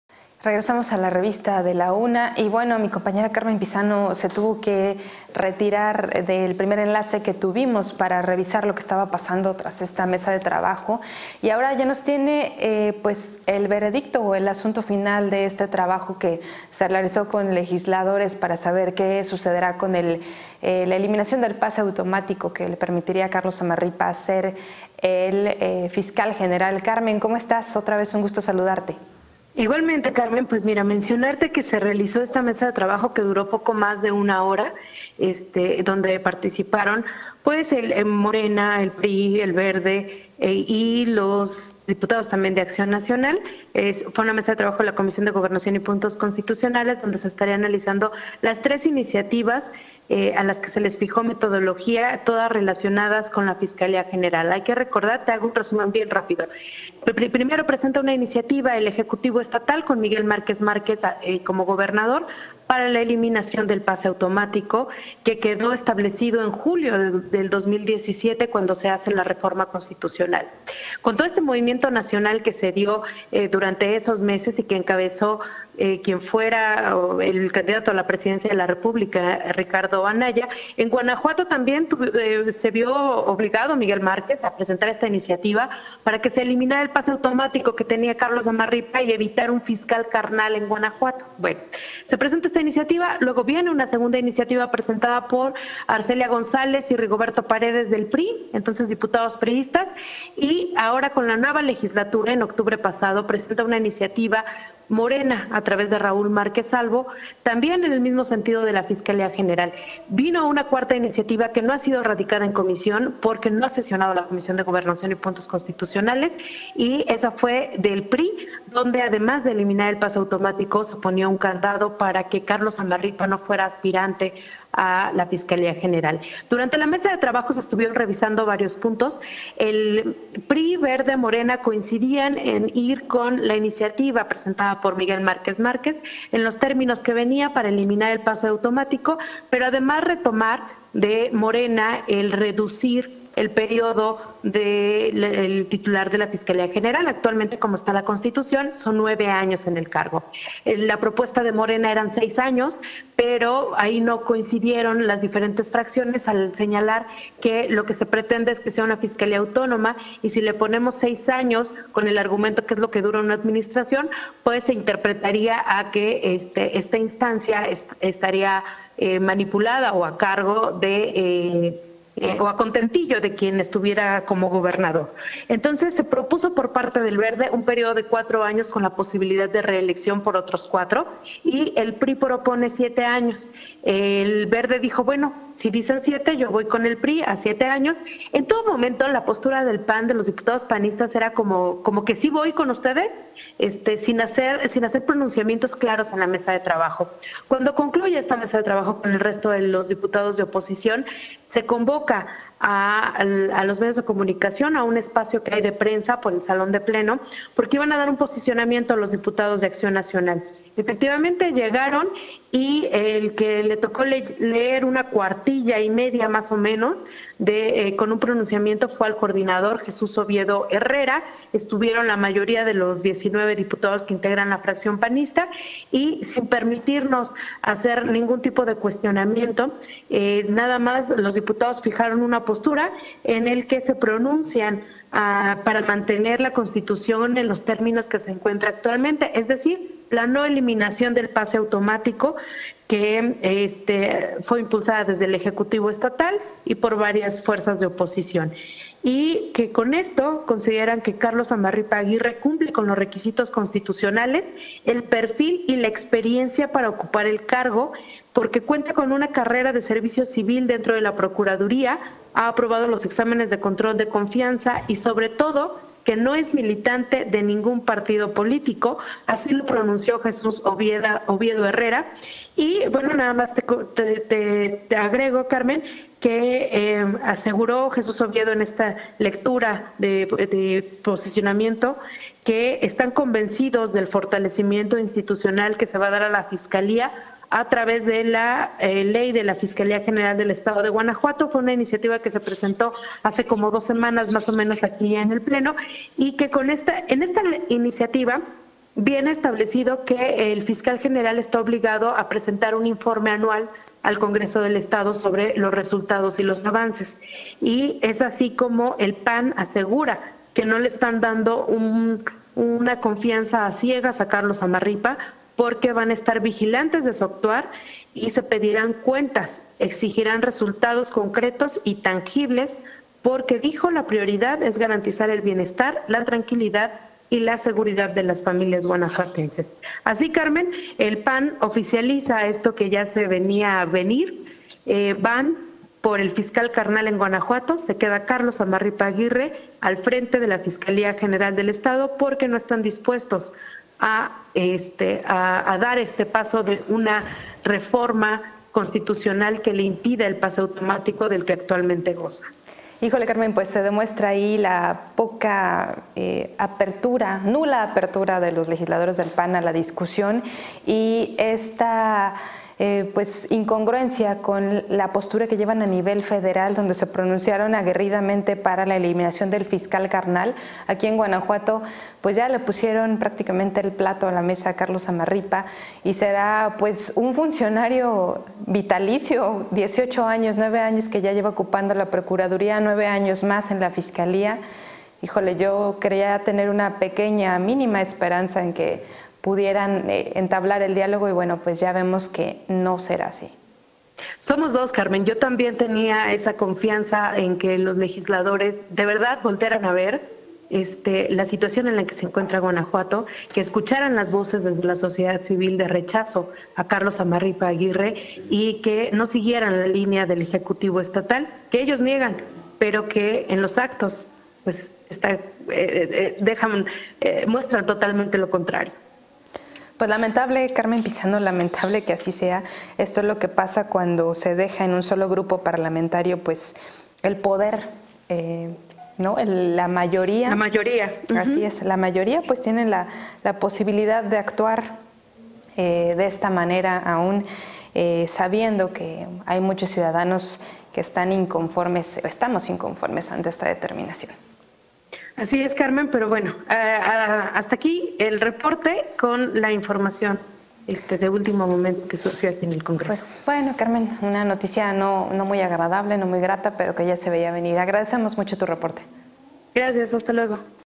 Zfreporte: Confirmado, PAN va por “fiscal carnal” en Guanajuato
Guanajuato, Gto. La fracción panista en el Congreso local hizo oficial el aval para que Carlos Zamarripa Aguirre pase de procurador de Justicia a primer fiscal general del estado, al manifestar que el procurador de justicia del estado que ha estado más de nueve años en el cargo cumple con todos los requisitos constitucionales, el perfil y la experiencia para ocupar el cargo por nueve años más, con la posibilidad de reducir el periodo.